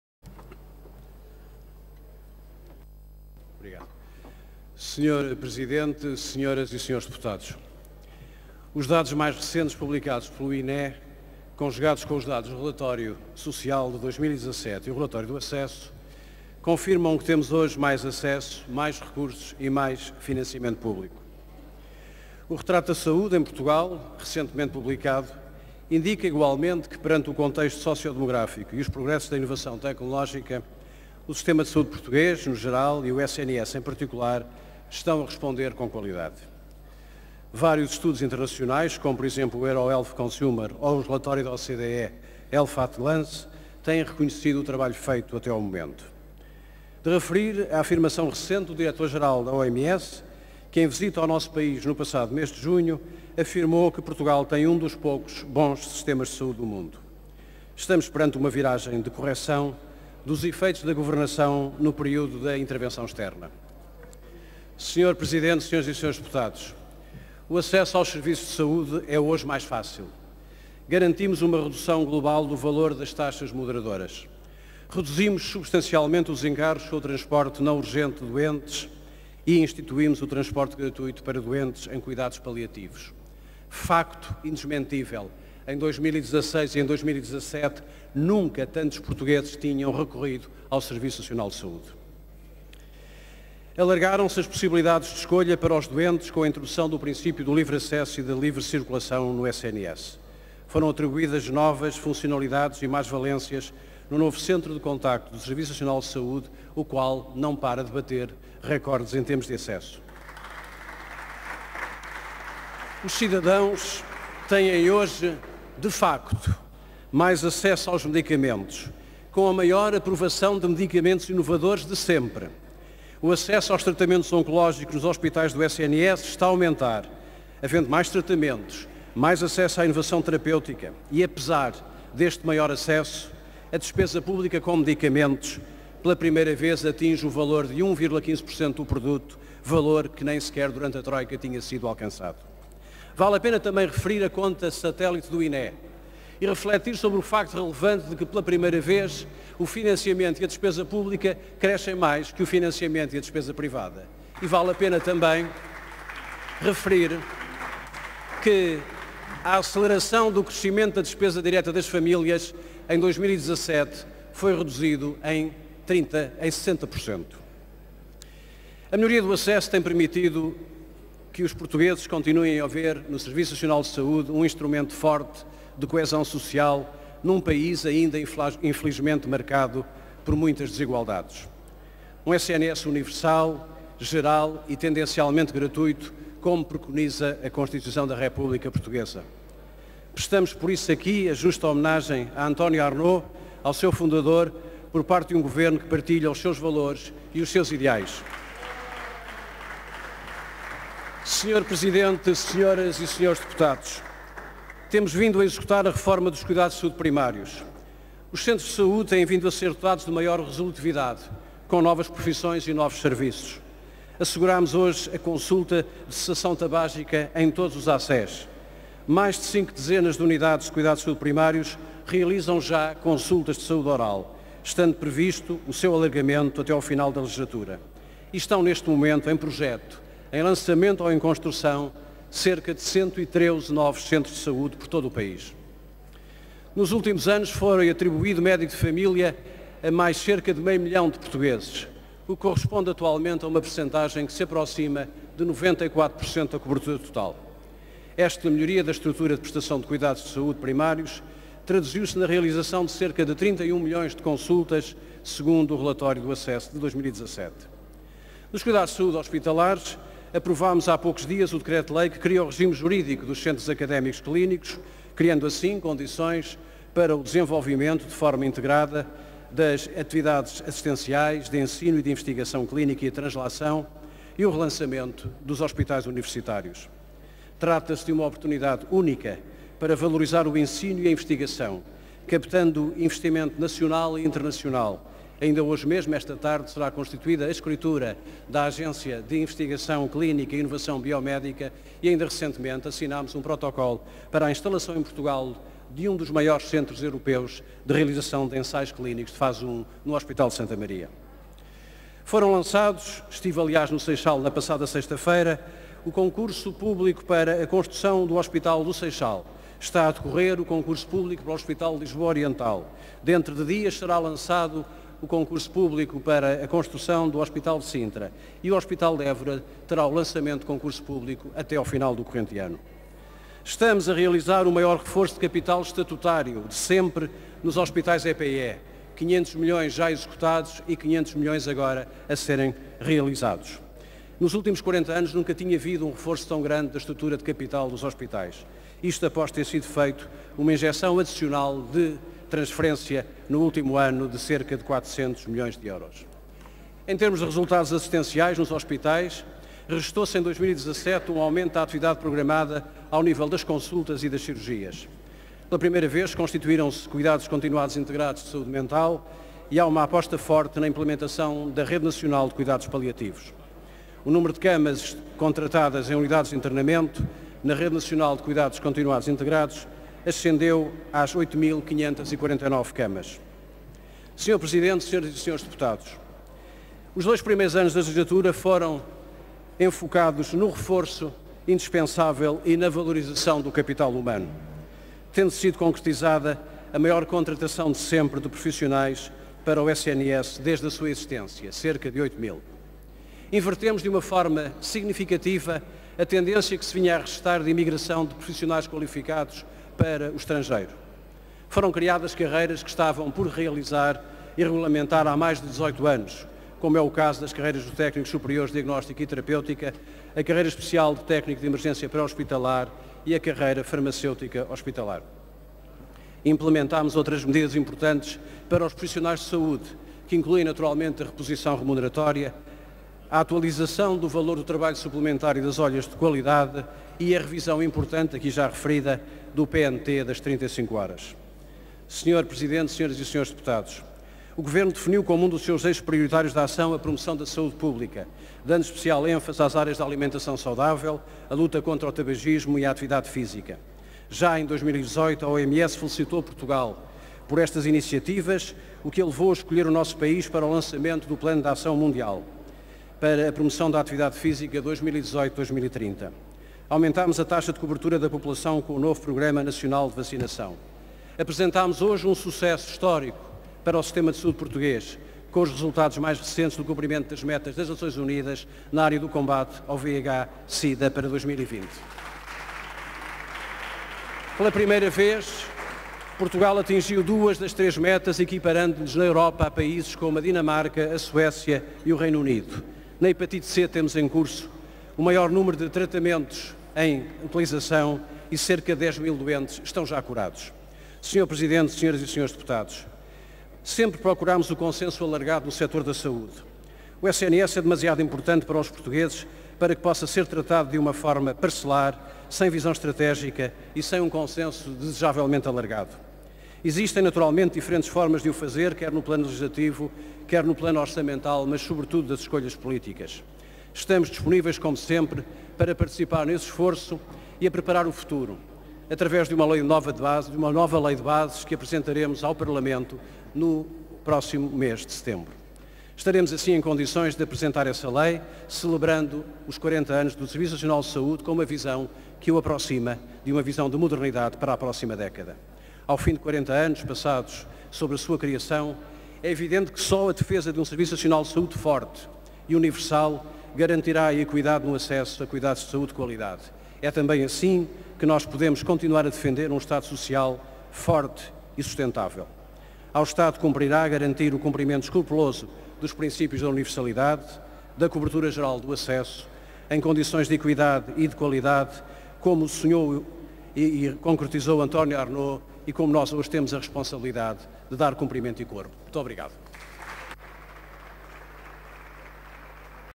05-07-2018 - Debate Parlamentar | Defesa do Serviço Nacional de Saúde | Adalberto Campos Fernandes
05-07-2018 | Intervenção do Ministro da Saúde Adalberto Campos Fernandes, no âmbito da Interpelação ao Governo (PEV) Defesa do Serviço Nacional de Saúde